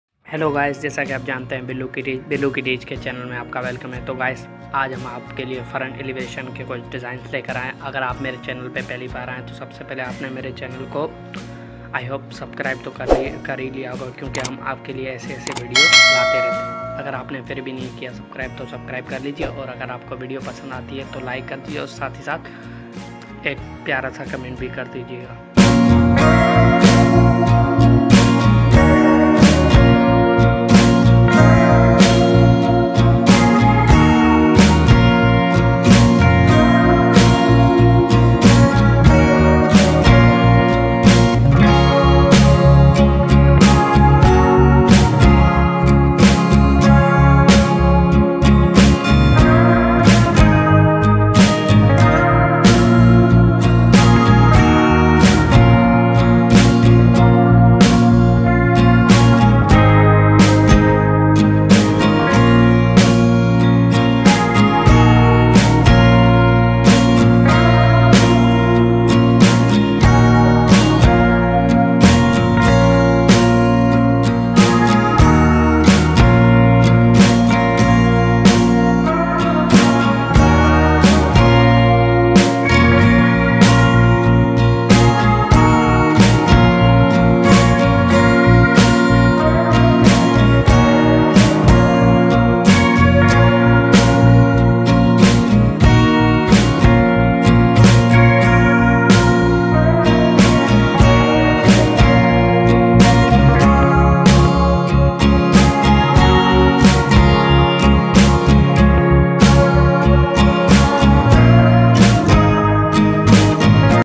Front elevation modern house !front elevation design India
Welcome to a world of modern architectural marvels! In this video, we explore stunning front elevation designs for houses in India. Discover the perfect blend of contemporary aesthetics and traditional charm as we showcase some of the most inspiring front elevation ideas. From sleek lines and innovative materials to spacious layouts and intricate detailing, this video will take you on a visual journey through the best in Indian home design. Whether you're an architect, a homeowner, or just someone with a passion for beautiful homes, join us as we unravel the secrets behind creating a modern front elevation that leaves a lasting impression. Don't miss this opportunity to get inspired and transform your home's facade into a work of art.

FrontElevation elevation design